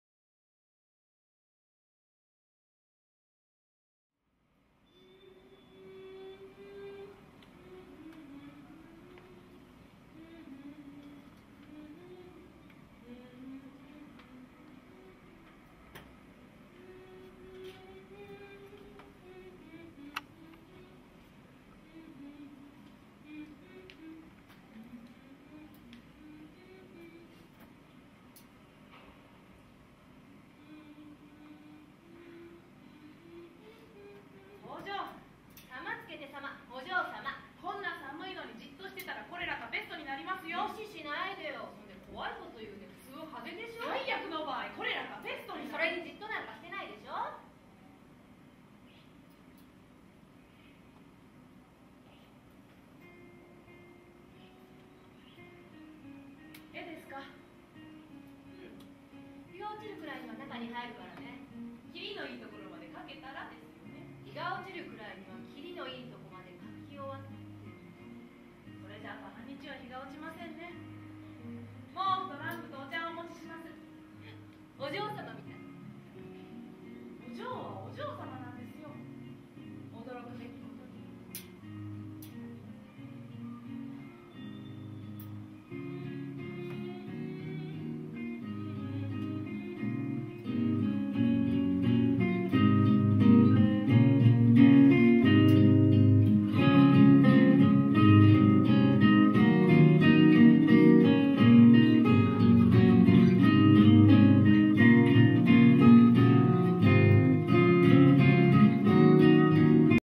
【キビるフェス2019】彗星マジック「ポストグラフ」PV
キビるフェス2019参加作品
彗星マジック「ポストグラフ」のPV(冒頭部分)です。